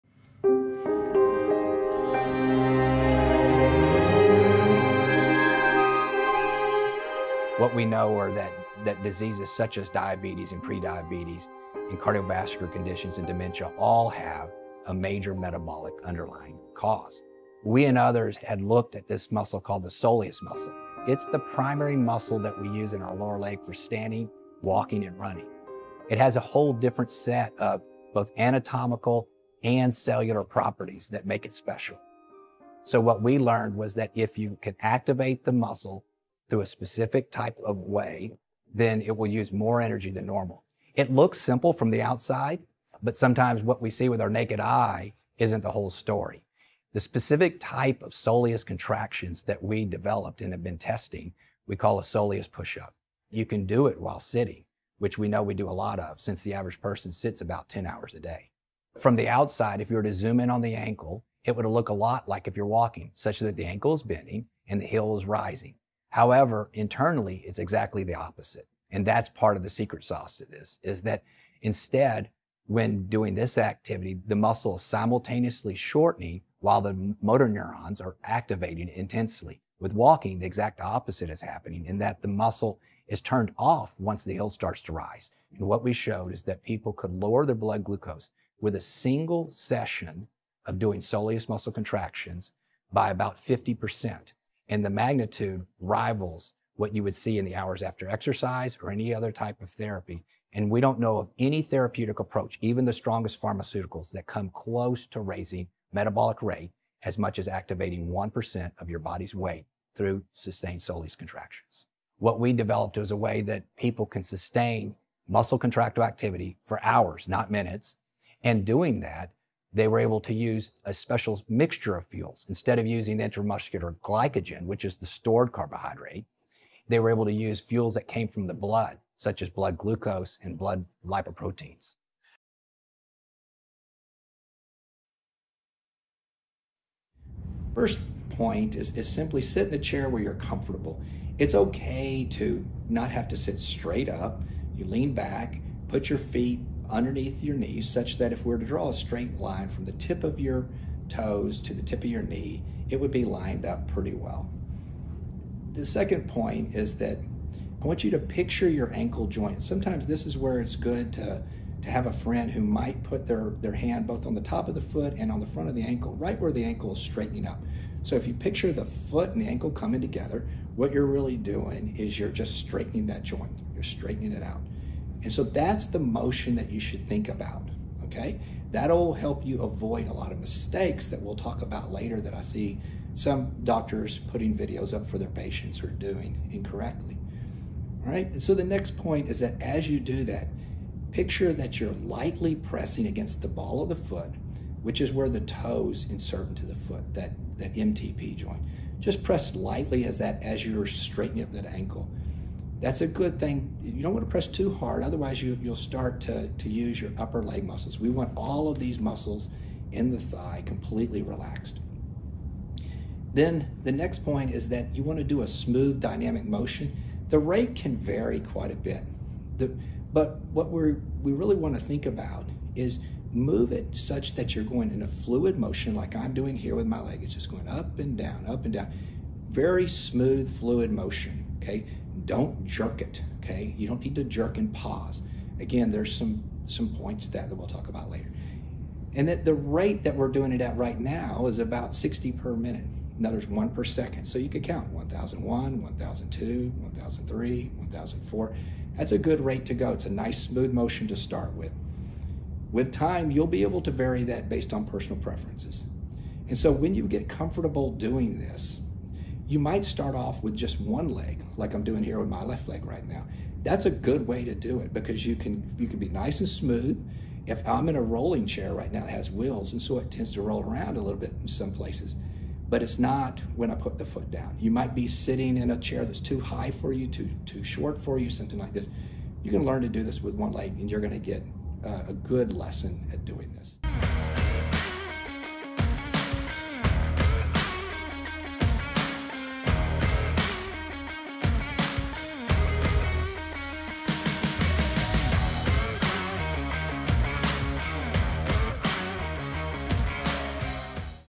The BEST Exercise To REDUCE Your Blood Glucose??
A simple exercise that shows reducing blood glucose over 50% in a science study.   The author Dr. Marc Hamilton explains the science behind why this simple movement works and how to do this exercise correctly step by step in this video.

A potent physiological method to magnify and sustain soleus oxidative metabolism improves glucose and lipid regulation

(Please sign up their free newsletter for more information)

🔶 Restore Our Cells Ability To Make NAD Like When We Were Young -